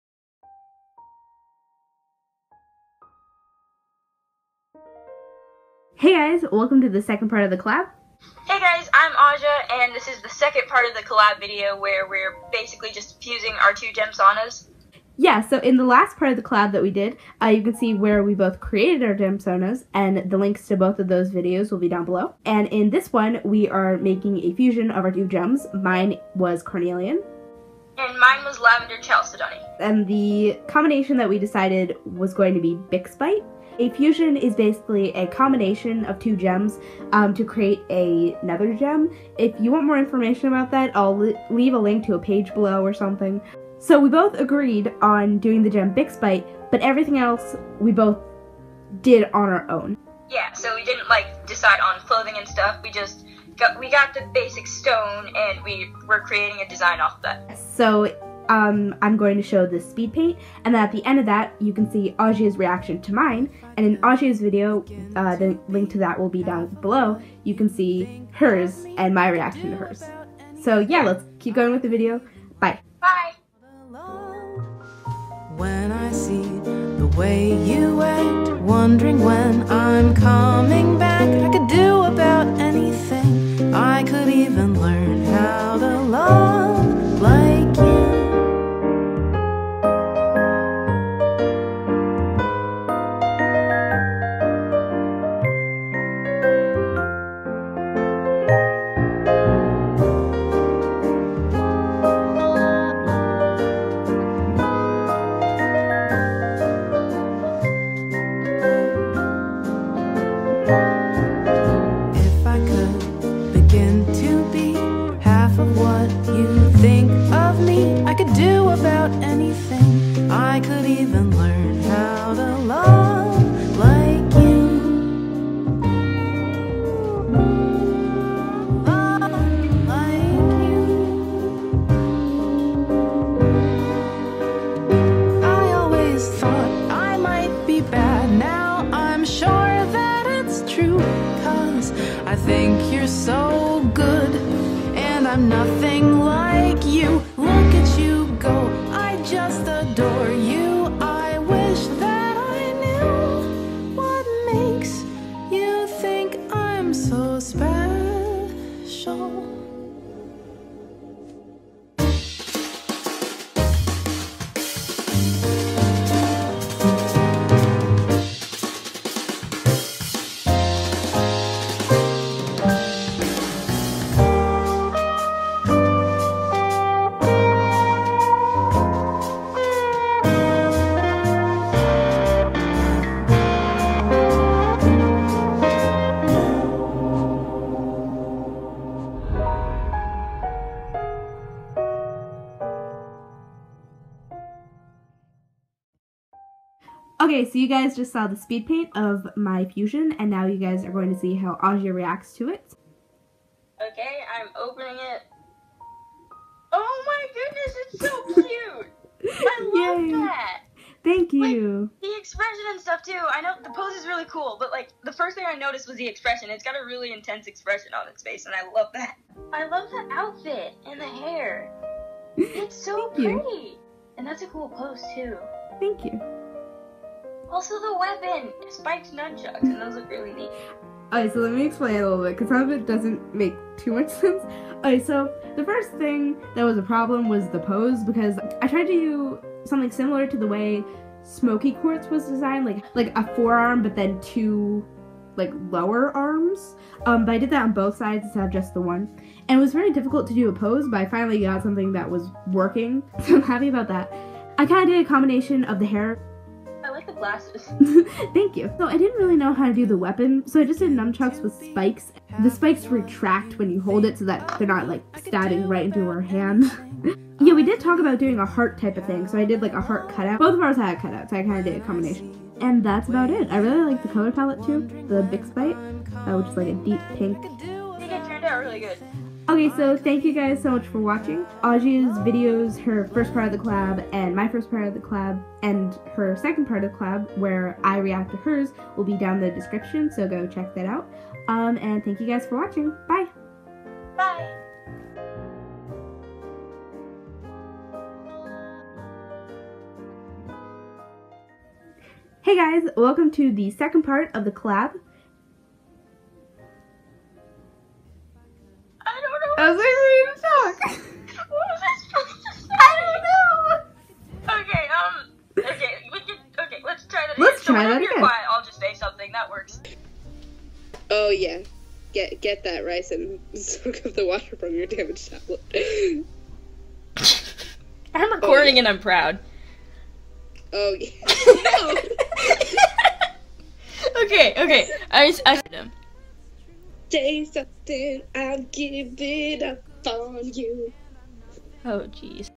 Bixbite • Steven Universe Gemsona Collab Speedpaint and Discussion
In one of my last videos, links below, I promised the second part to the collab with Ausia and I and here it is! In the last part of the collab, Ausia and I created our gemsonas and in this part we created a fusion of them! Ausia's social medias and videos are below as well as my first part of the collab. Enjoy!